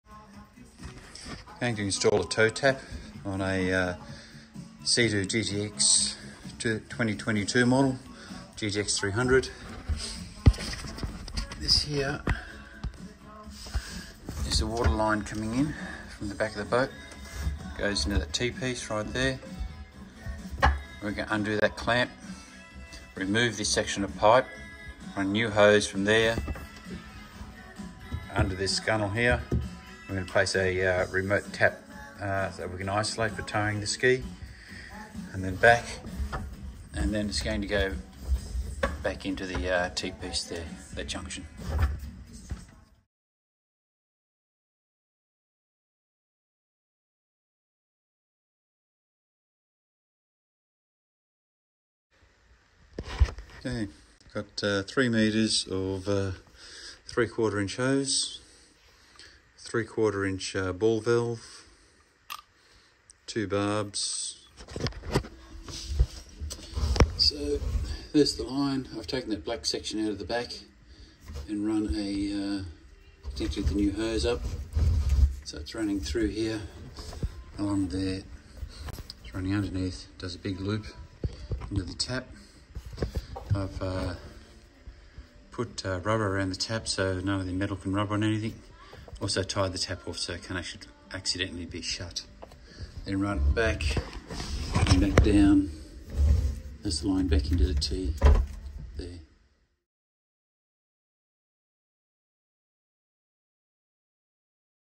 Seadoo GTX300 2022 Tow Tap Install
Adding an accessible tow tap to isolate water from entering the engine to the ski if it’s under tow.